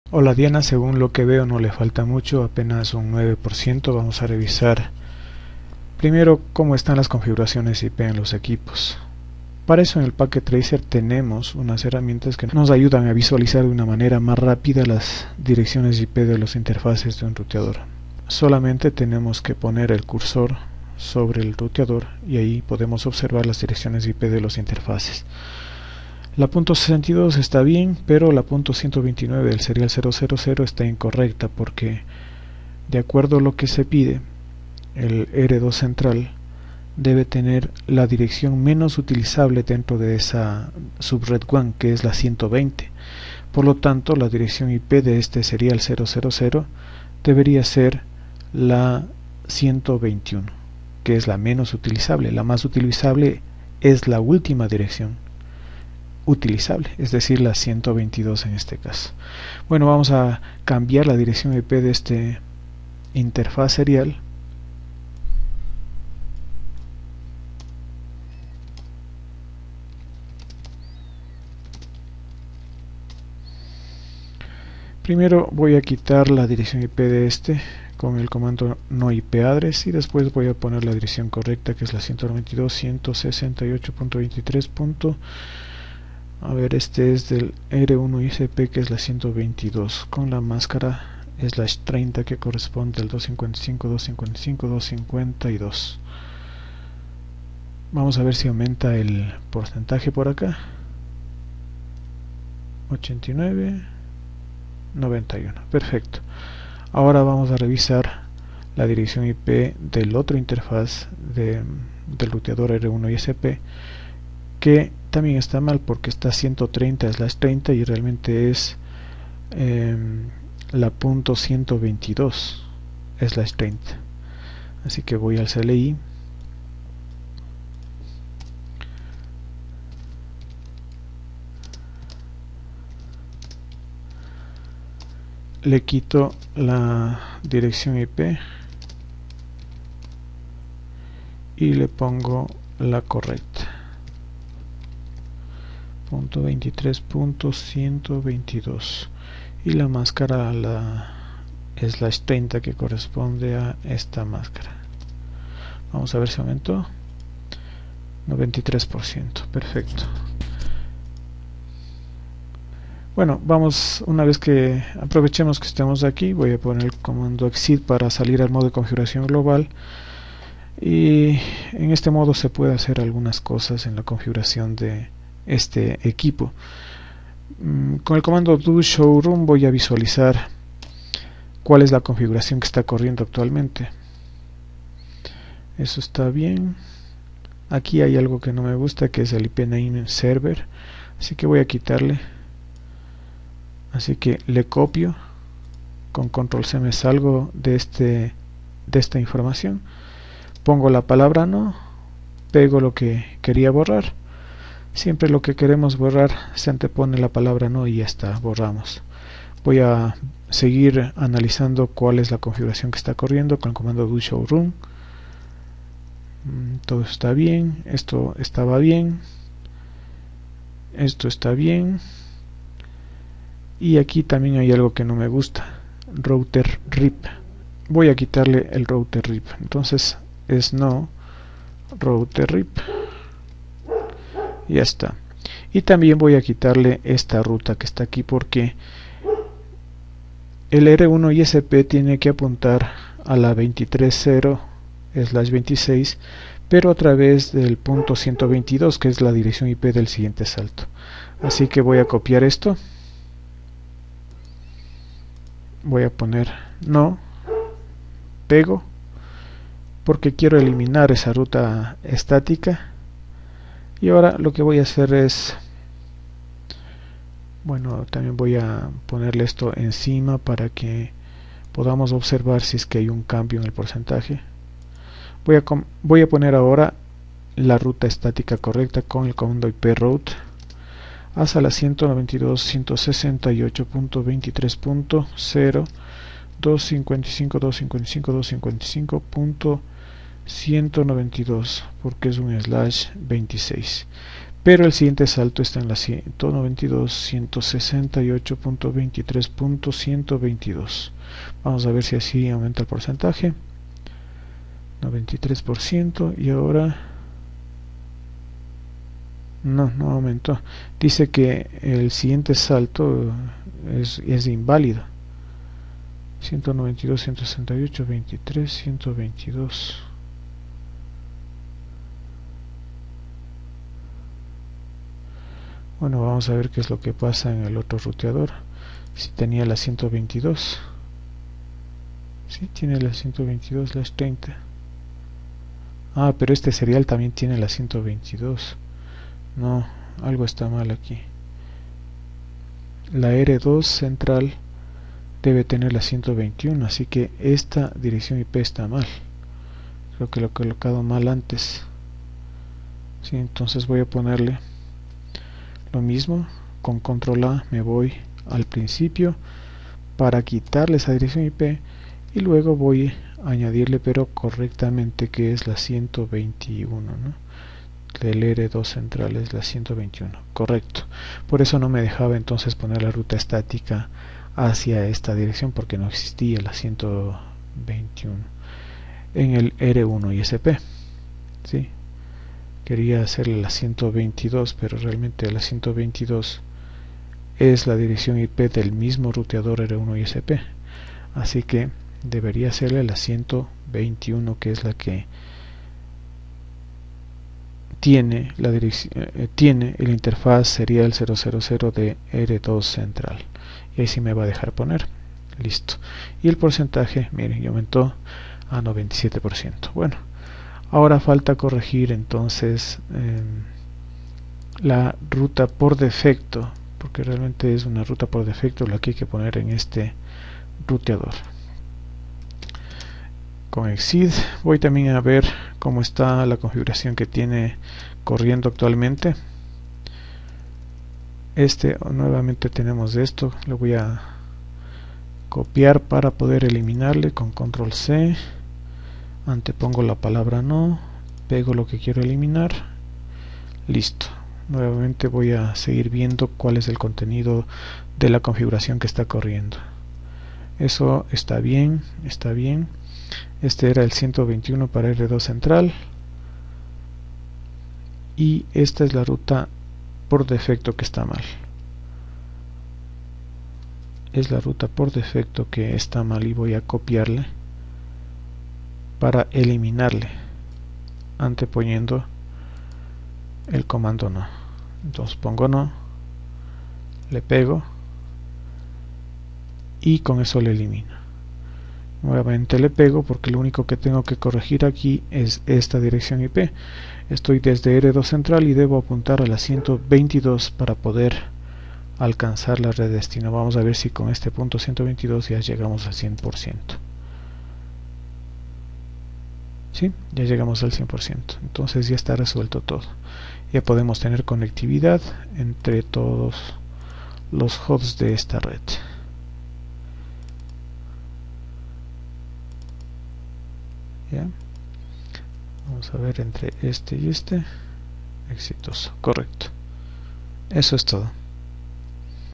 Solución de problemas PT-6.8.1
Práctica de laboratorio 6.8.1 hecha en Packet Tracer. Solución de problemas de direccionamiento y enrutamiento.